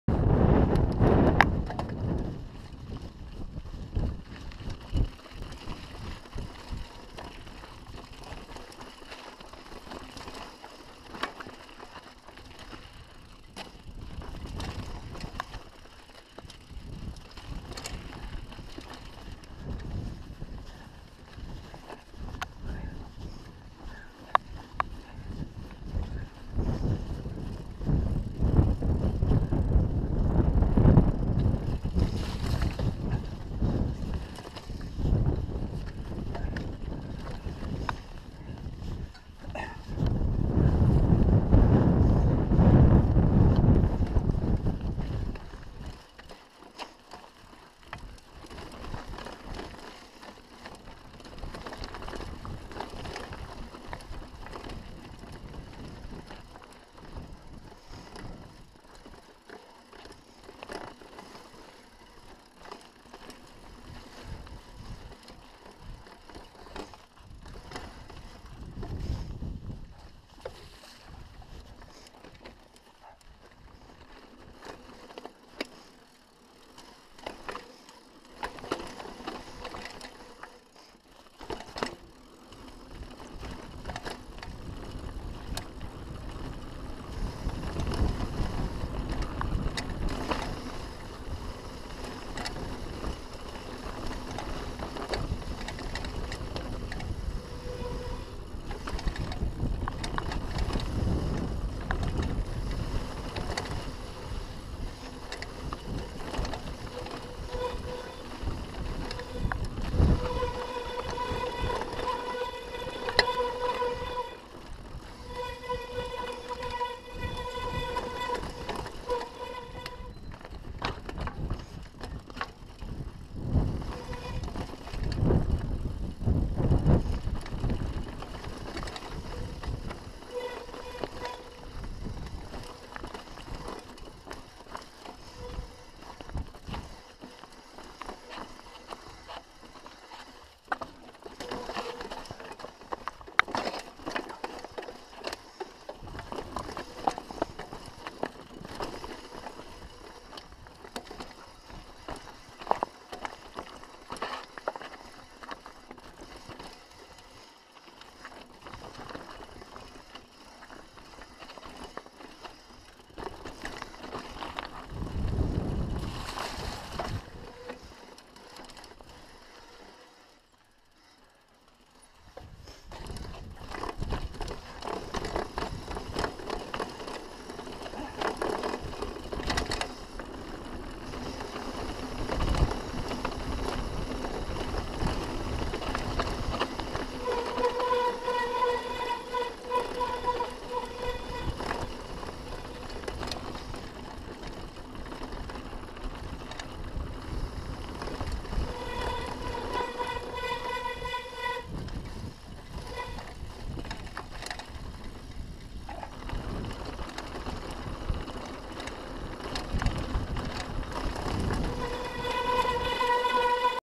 Peak District MTB - Whinstone Lee Tor Eastside Descent
Thhs descent has is all, great views, moorland singletrack, ruts, rocks, watersplashes and flagstones. I rode this in pretty much perfect conditions. However the top part of the route can be very muddy and if it has snowed - well good luck.